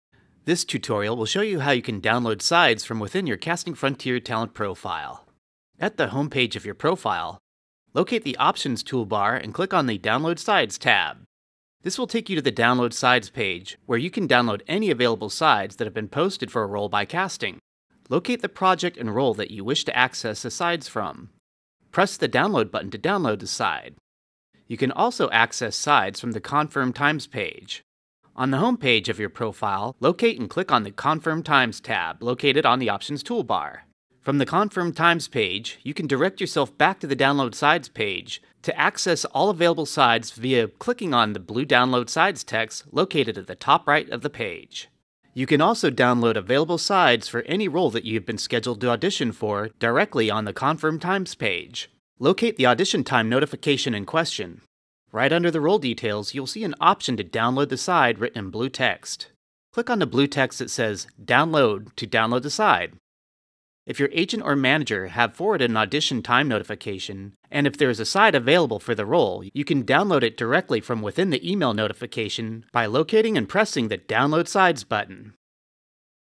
Downloading Sides
This tutorial will walk you through the steps for Downloading Sides via your Casting Frontier Talent Profile.